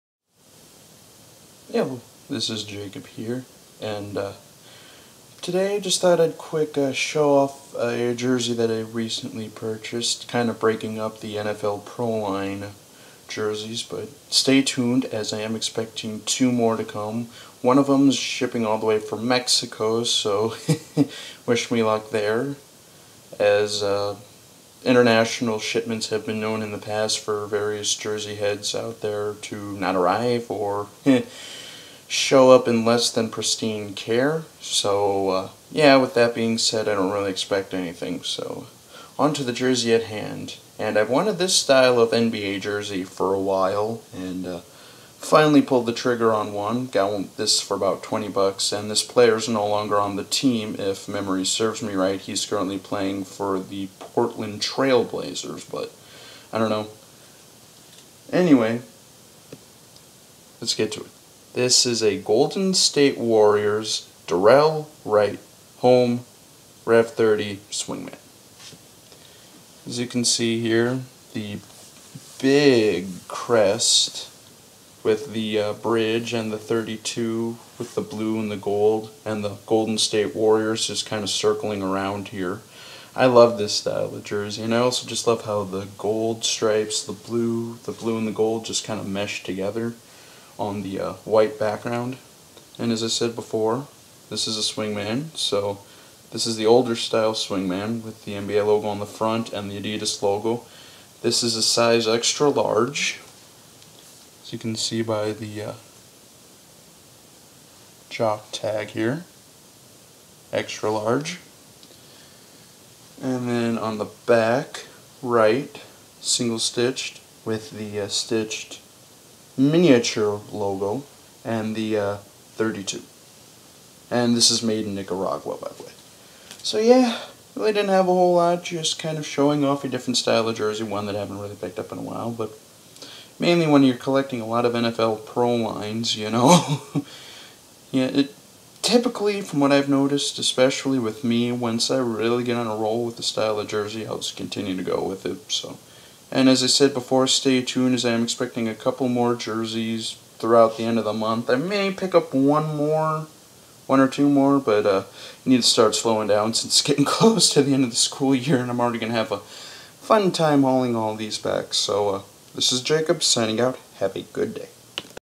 NBA Swingman Pickup
My latest Swingman, Golden State Warriors Brandan Wright jersey XL